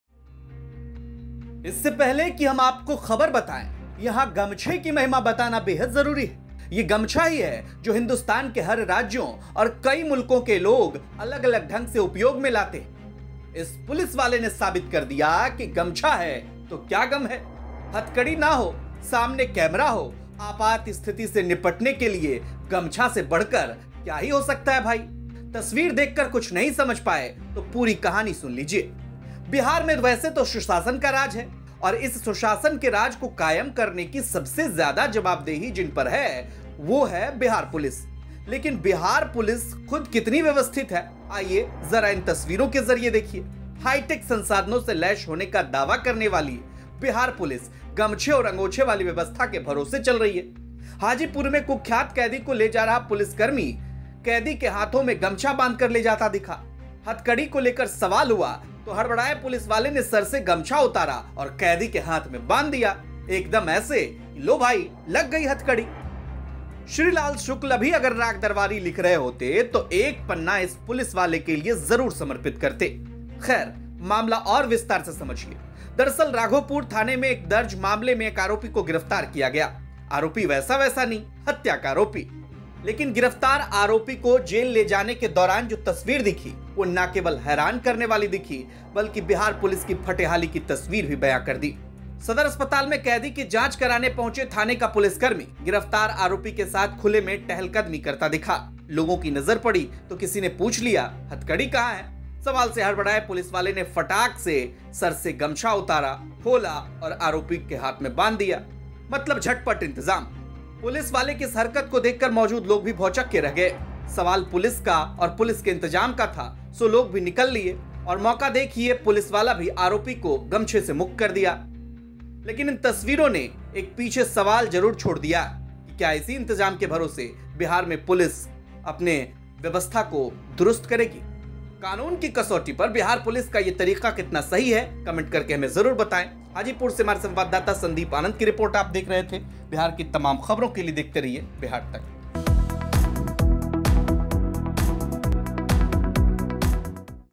Ajab gazab: हथकड़ी पर हुआ हुआ सवाल तो हड़बड़ाए पुलिसवाले ने गमछे से बांध दिया कैदी का हाथ
हाईटेक संसाधनों से लैश होने का दावा करने वाली बिहार पुलिस गमछे और अंगोछे वाली व्यवस्था के भरोसे चल रही है हाजीपुर में कुख्यात कैदी को ले जा रहा पुलिसकर्मी कैदी के हाथो में गमछा बाँध ले जाता दिखा.